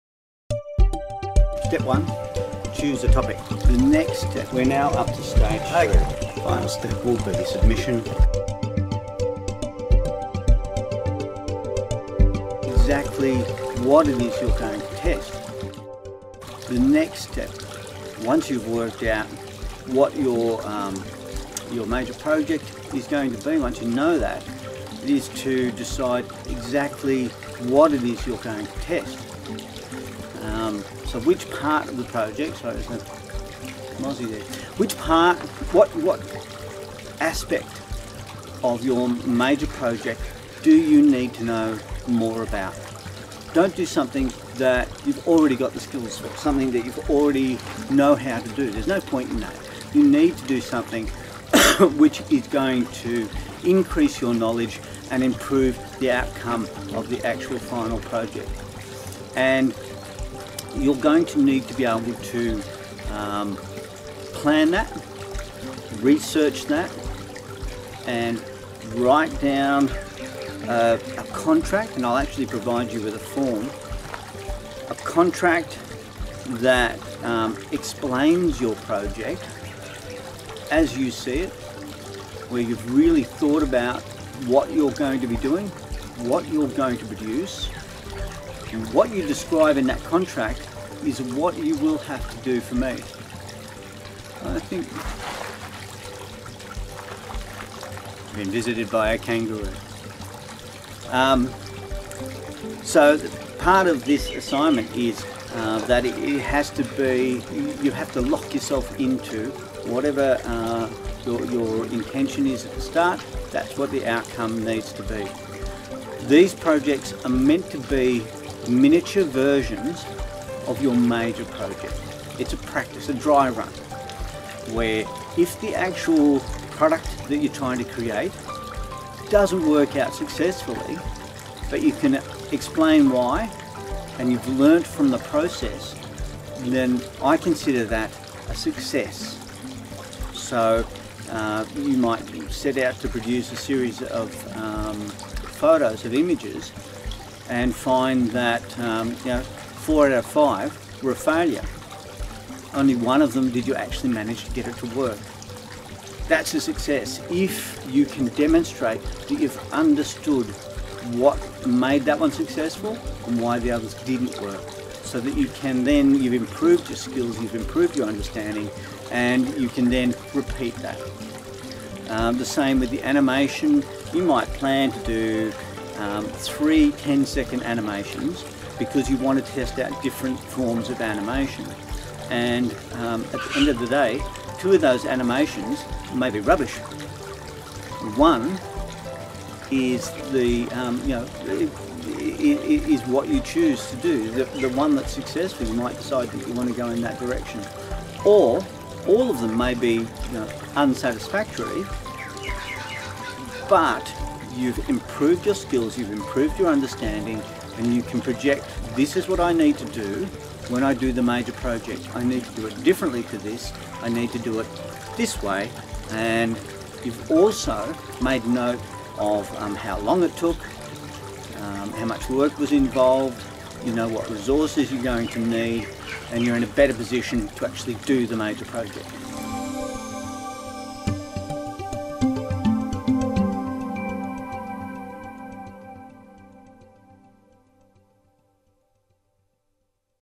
Media AOS 3 2 Part 2 Think about what you need to know
In this tutorial, I explore the question, 'what it is you need to learn, to help you achieve the best major project you are capable of?'